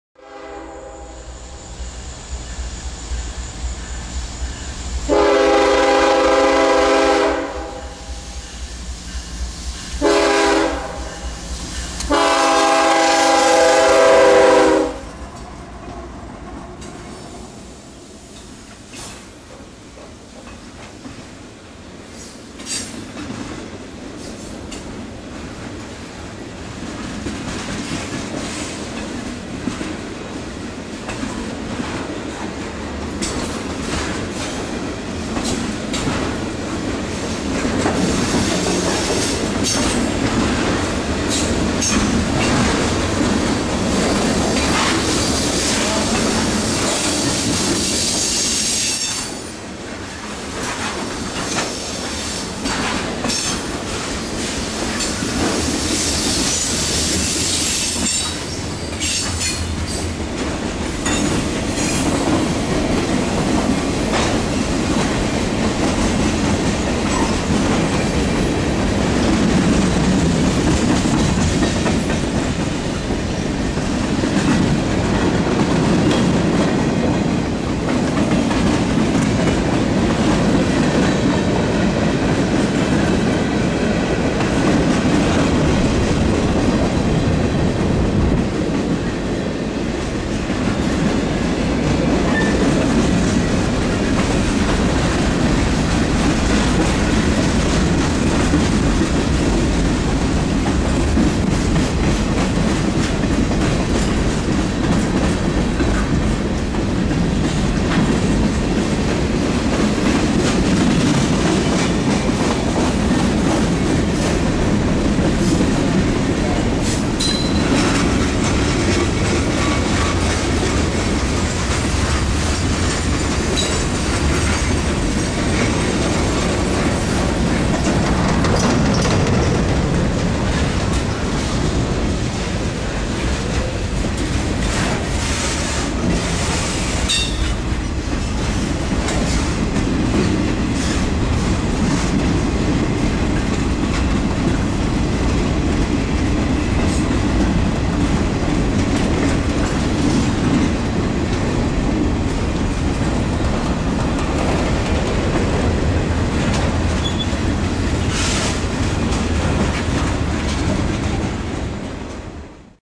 WB CSX Q-409 manifest 8/19/09
On August 19 2009 CSX Q-409 rolls slowly west through Piscataway NJ on the CSAO Lehigh Valley Line with a few stinkers from the trash cars.

Info below.

Comment and Rate =D